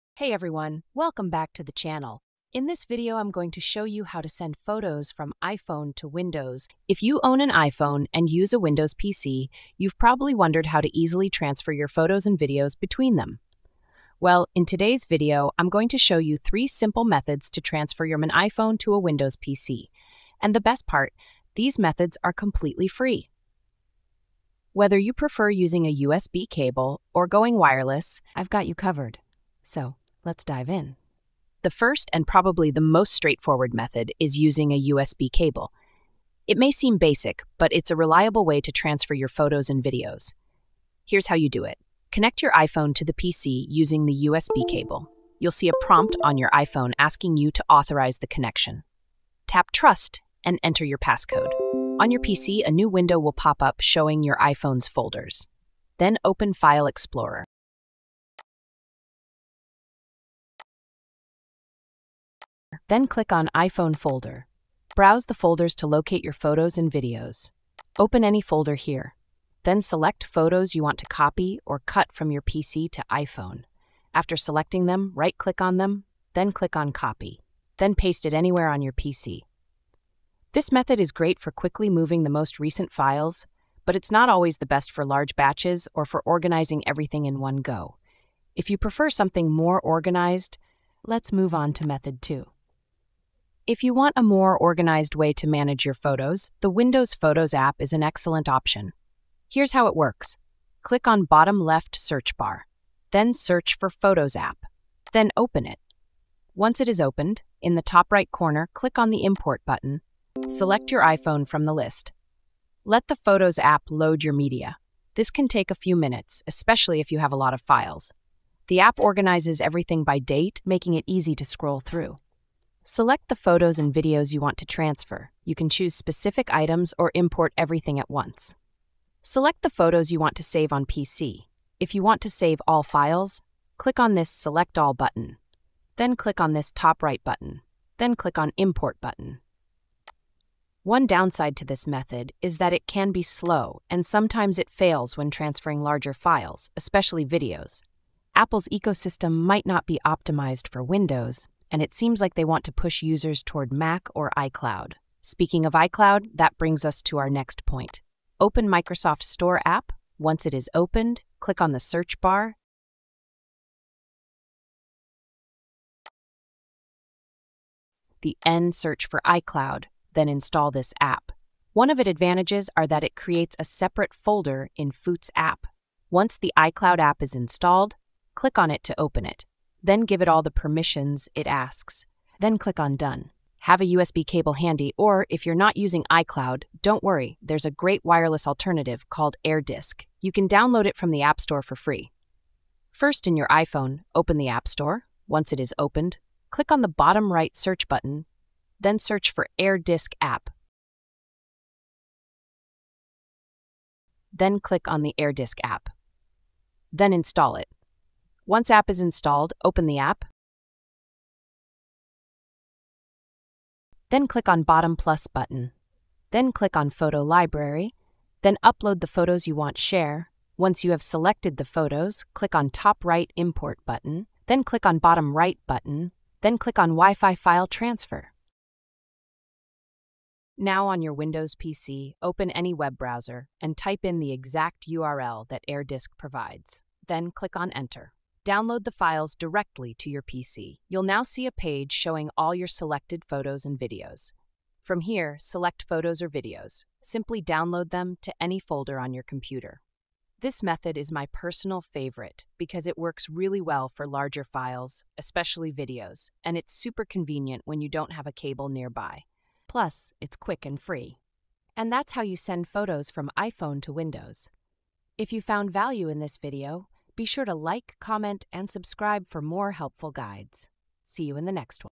How To Send Photos From iPhone To Windows | 3 FREE Methods To Transfer From iPhone To PC & Laptop
Learn how to send photos from iPhone to Windows in this easy step-by-step guide! Whether you're looking to transfer pictures from iPhone to Windows for backup or simply save your photos from iPhone to your computer, we've got you covered. Discover different methods to transfer images effortlessly without losing quality. Perfect for anyone wanting to free up space or organize their iPhone photos. Watch now and make the process simple!

💭 Have a question about this tutorial? You Can Ask Me In The Comments